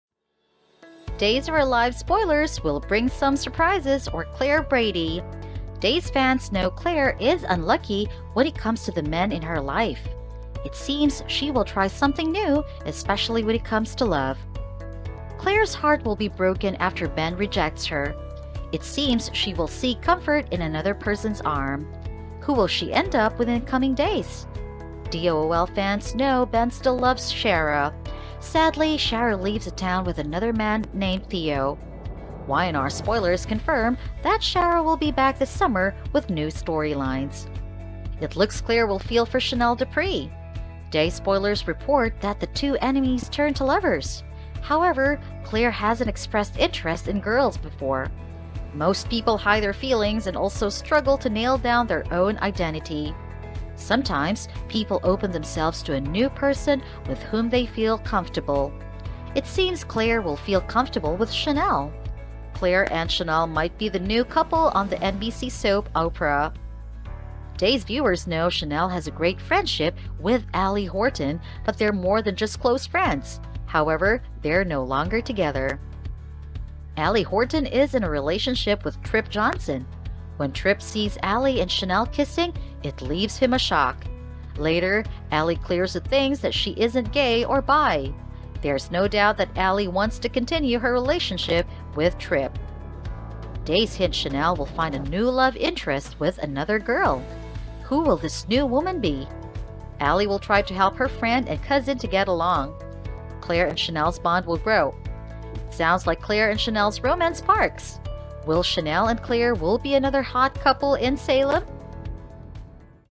Days Of Our Lives Spoilers: Ben Rejects Claire, Romantic Rebound To Chanel
Days of Our Lives spoilers say some surprises are in store for Claire Brady. Claire's heartbroken after Ben rejects her. She will seek comfort in another person. Days fans know Claire is unlucky when it comes to the men in her life. Claire never expresses her interest in girls before. It seems she will find a love interest but with a woman. Claire will fall for Chanel and end up being a couple.

Thanks for coming by our channel!
We deliver Days of Our Lives Cast Updates & Spoilers here!